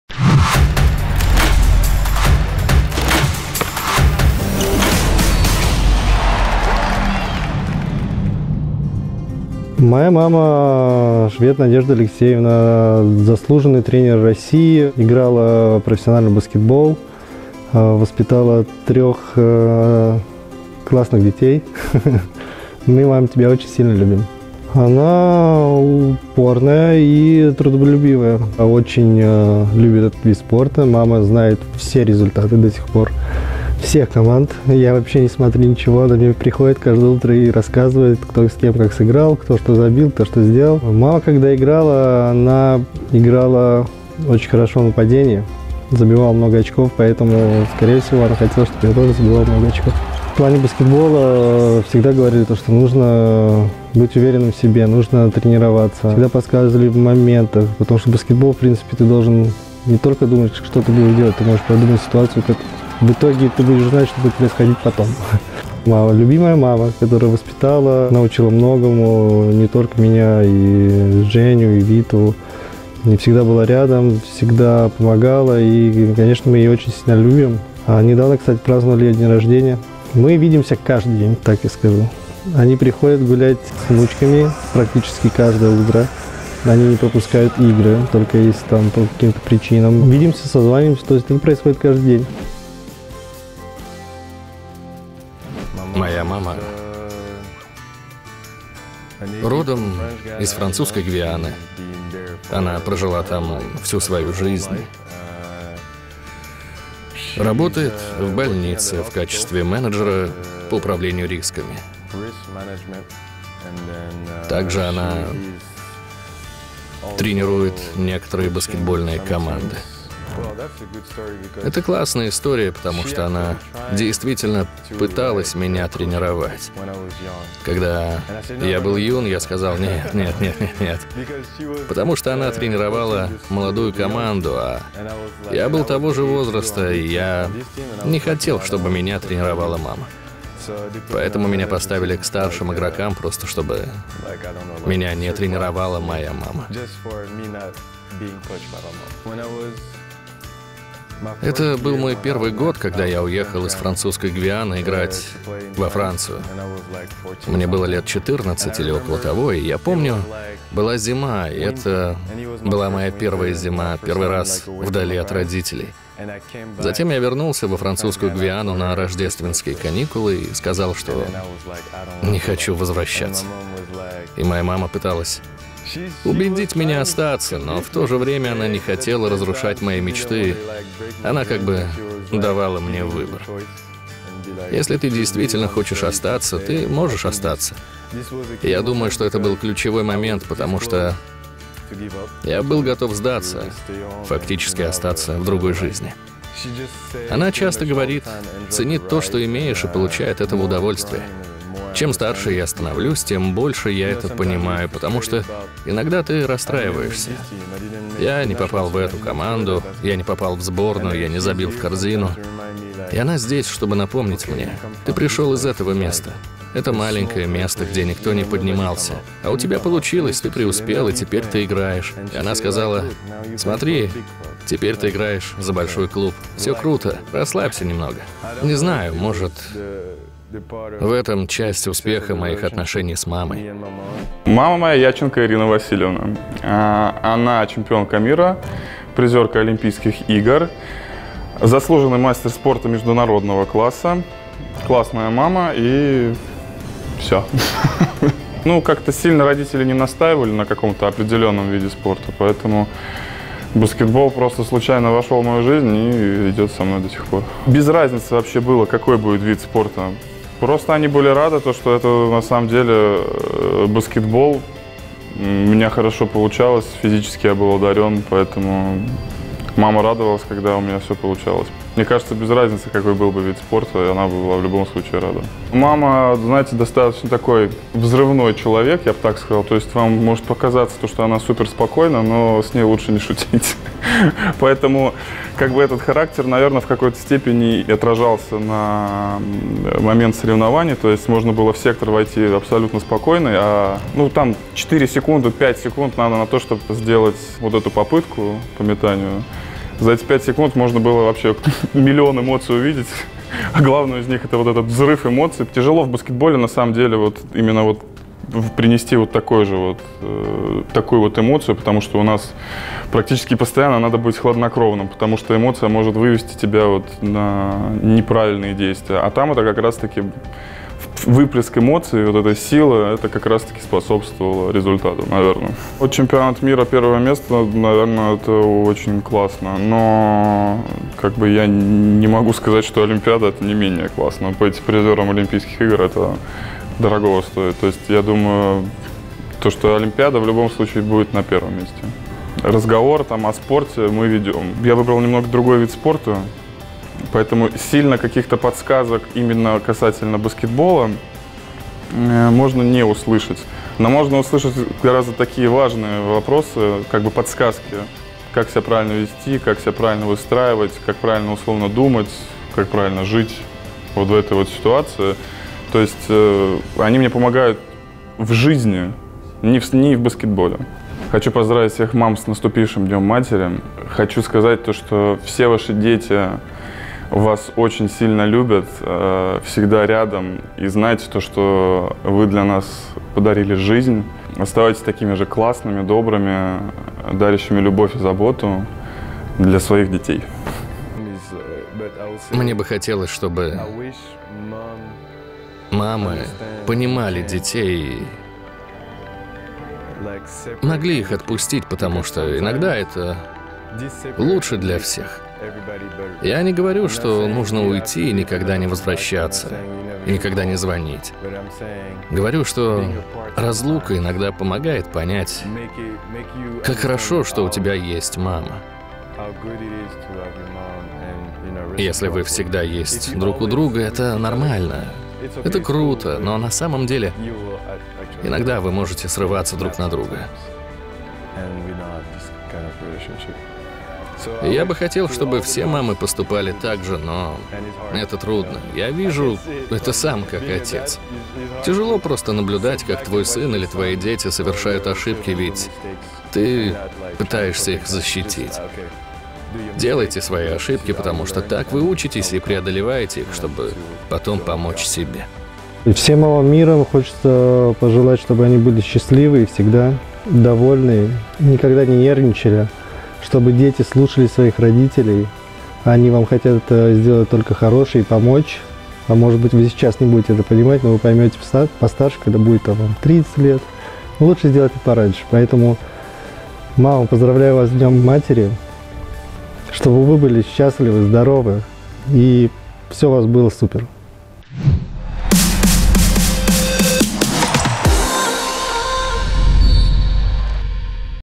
Поздравление игроков ПБК ЦСКА с Днем Матери
Дорогие мамы, наша главная поддержка и опора! Поздравляем вас с прекрасным праздником - Днём матери!

Алексей Швед, Антон Астапкович и Ливио Жан-Шарль поздравляют всех мам и рассказывают о своих: их спортивном пути и родительной поддержке